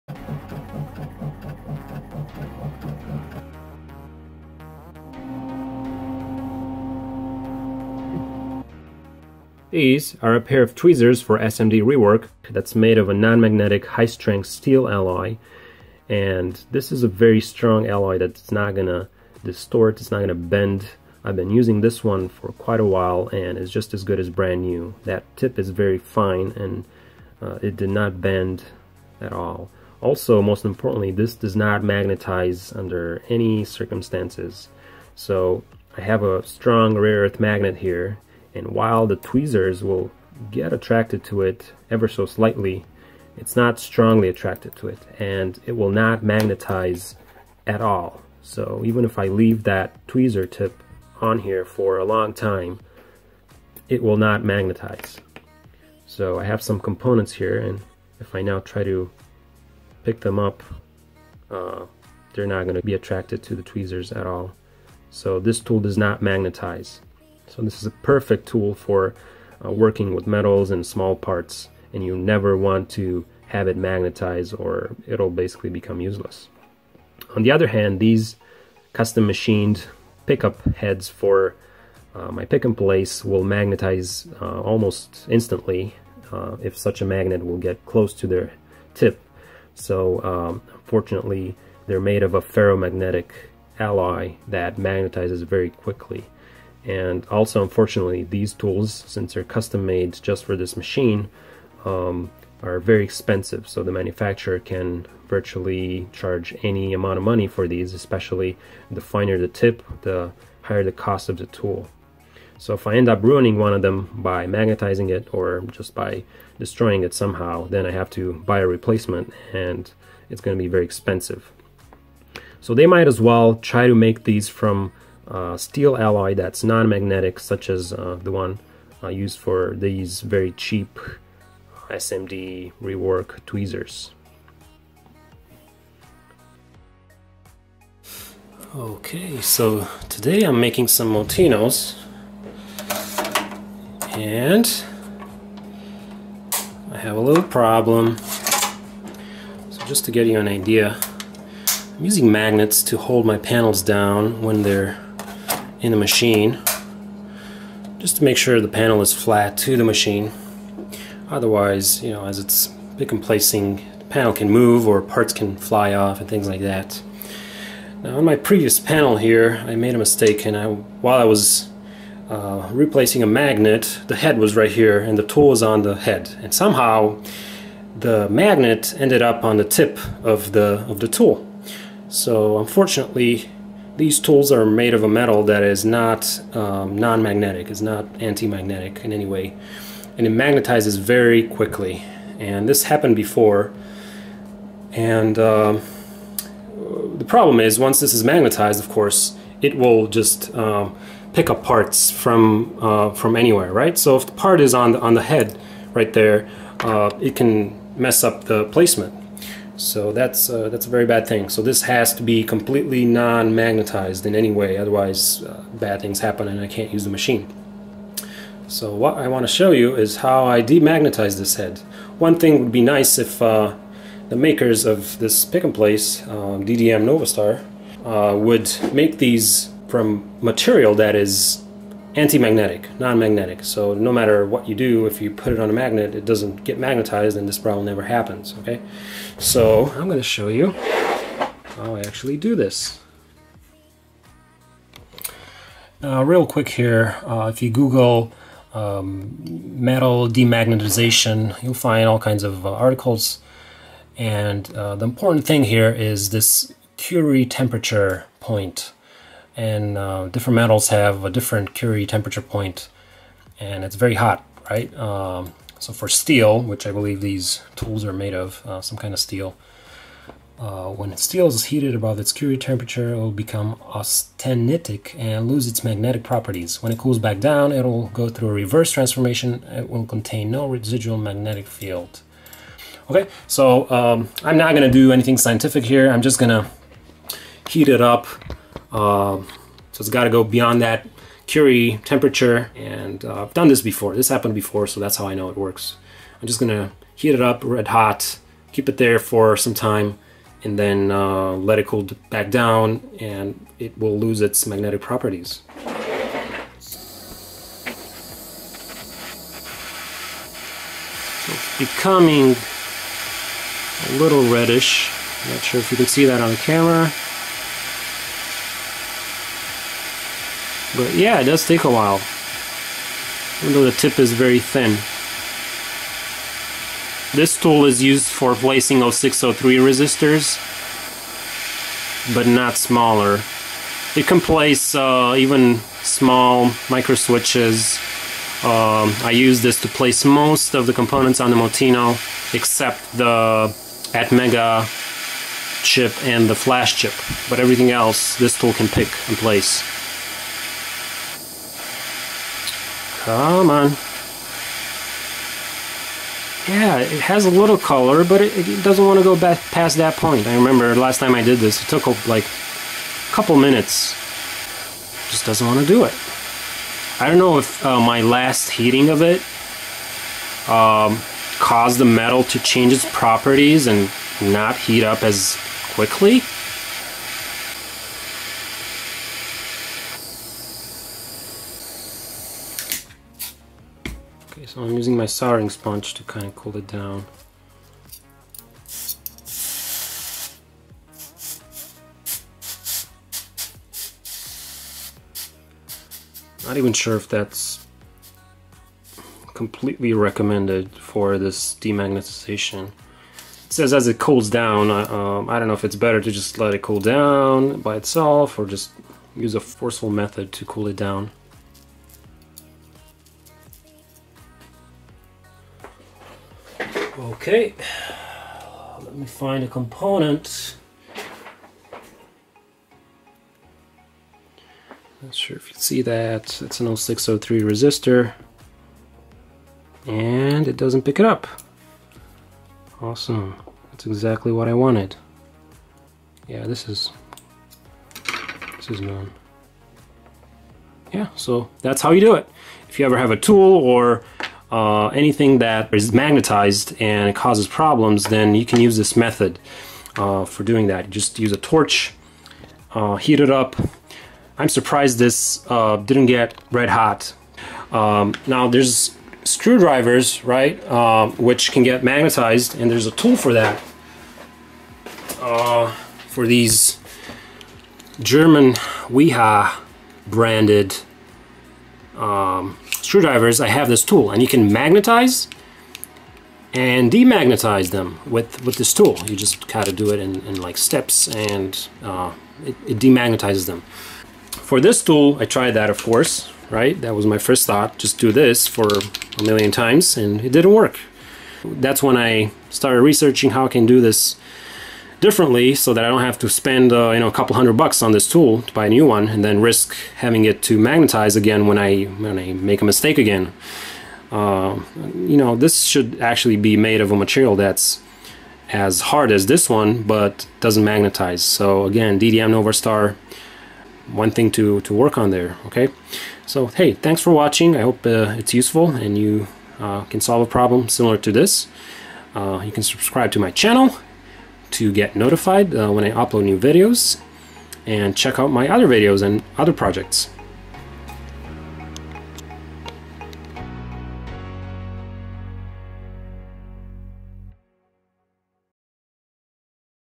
Pick & Place Nozzle Demagnetization
My DDM Novastar LE40-V pick & place machine nozzles are made from a ferromagnetic material which causes them to easily magnetize.
I show my workaround to getting these very expensive nozzles demagnetized.